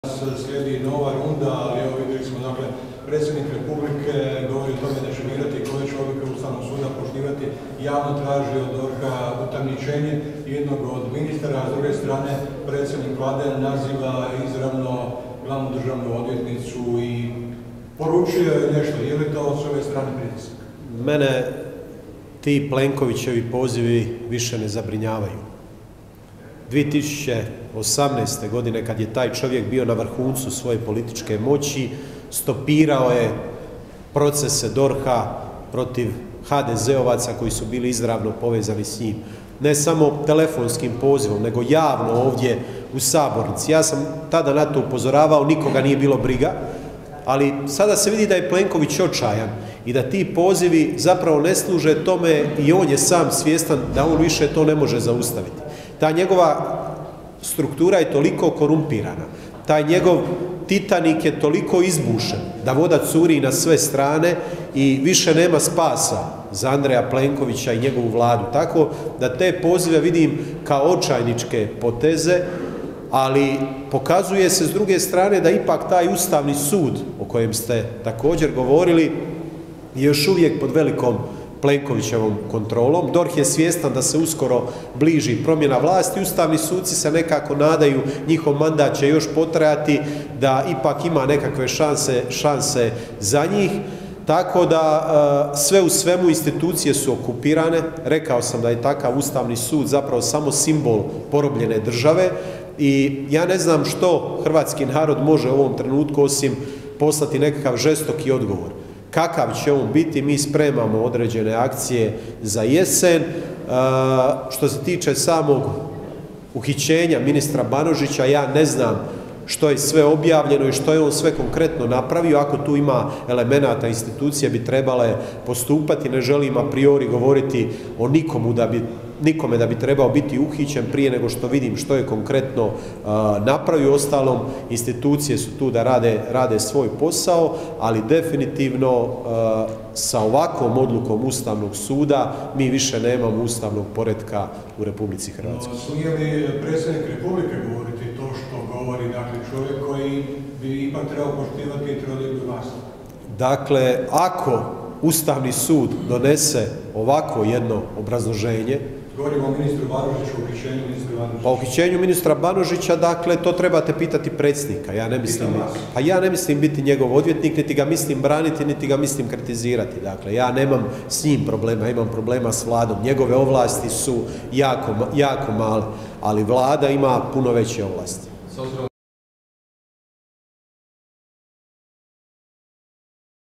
Grmoja: Plenković je očajan, nema spasa za njega i njegovu Vladu
Na konferenciji za medije održanoj u Saboru zastupnici Mosta Zvonimir Troskot i Nikola Grmoja komentirali su aktualne teme. Grmoja o Andreju Plenkoviću koji je očajan.